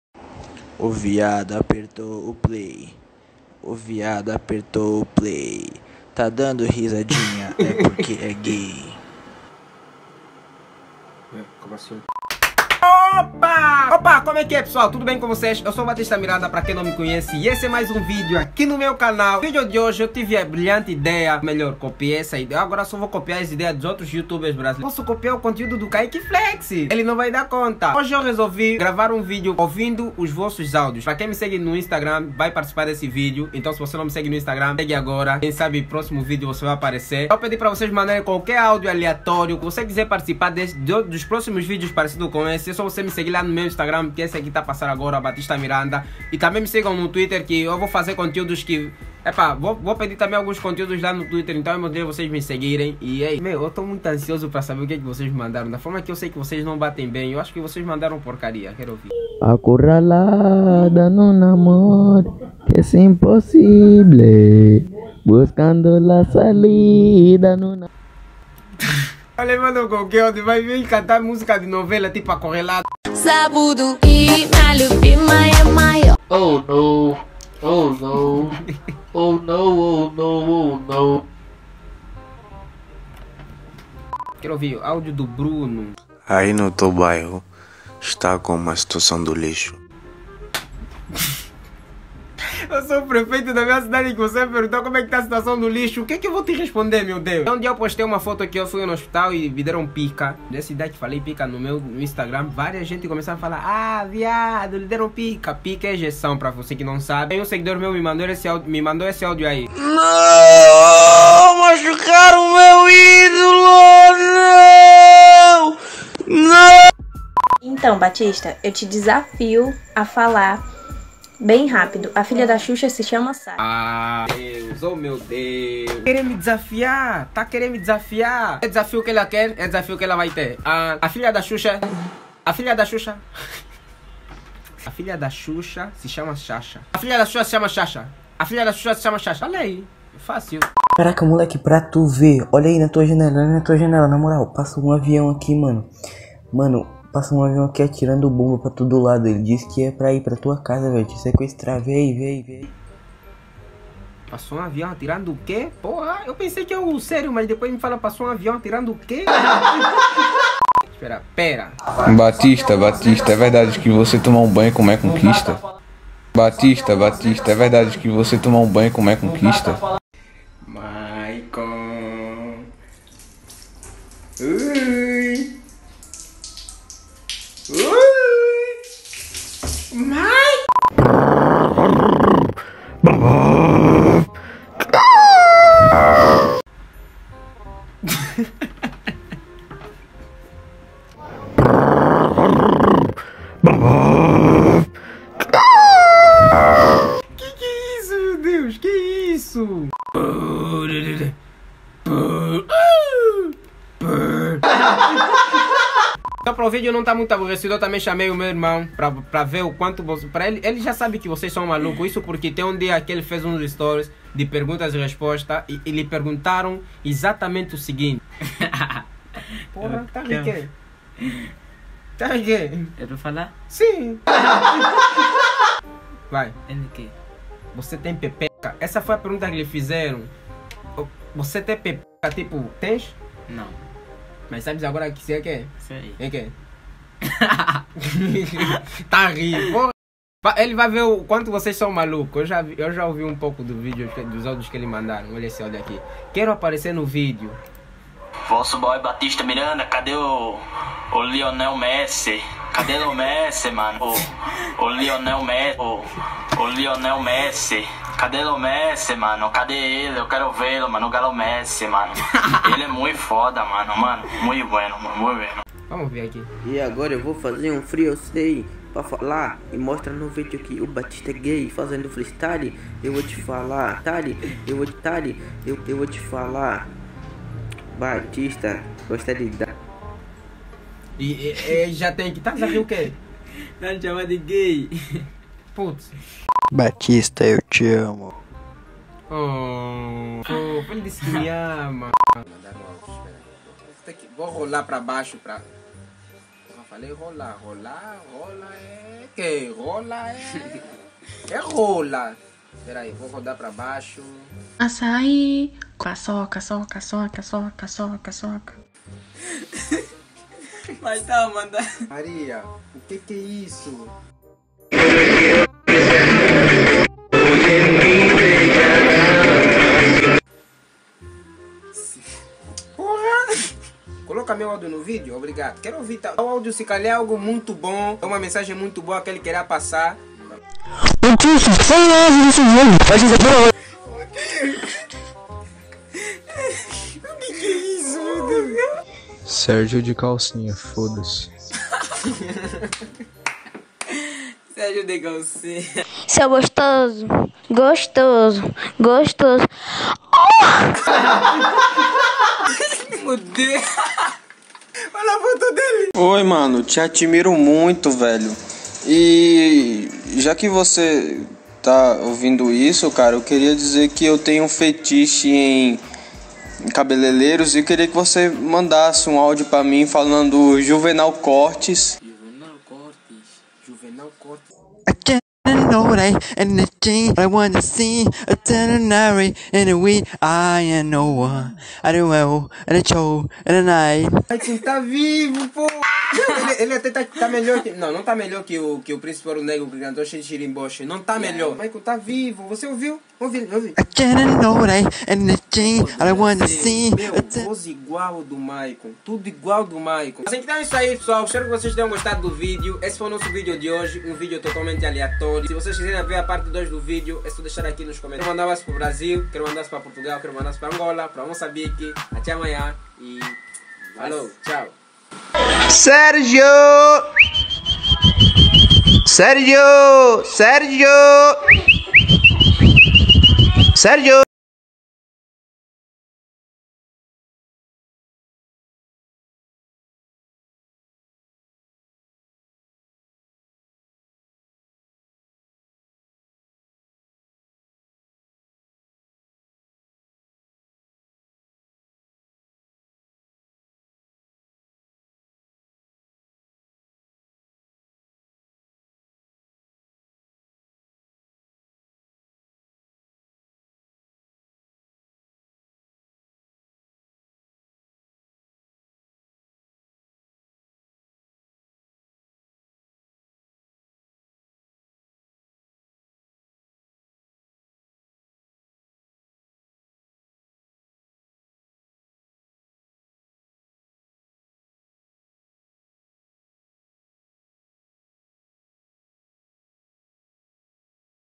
Ouvindo áudios estranhos dos INSCRITOS c/ meu irmão
ajude o canal ai Seja membro :

Se voce gostou do video não se esqueça de deixar o seu like.